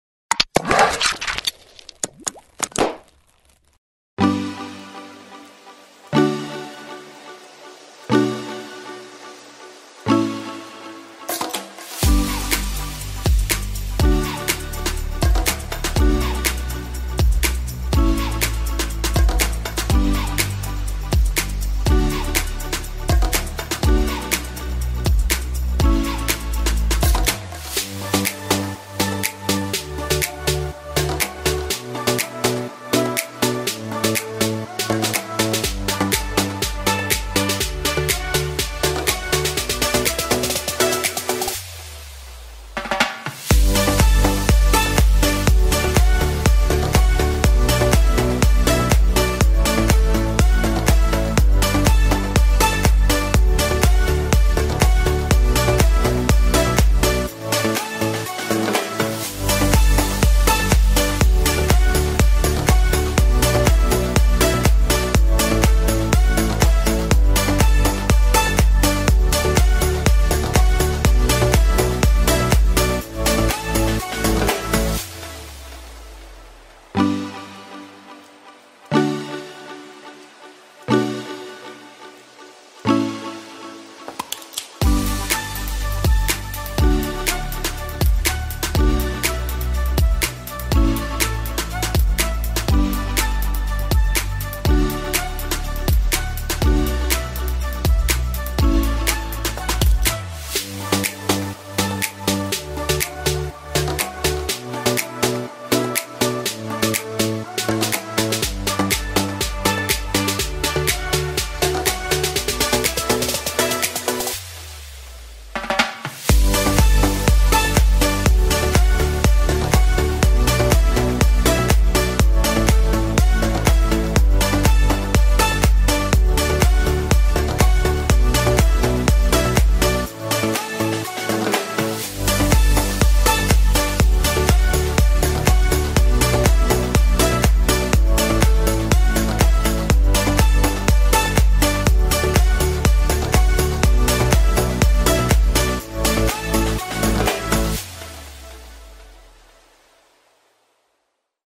Designing a Logo for PUBG Clan | Adobe Illustrator
In this video I design a Logo for PUBG clan of my sister. I hope you like it.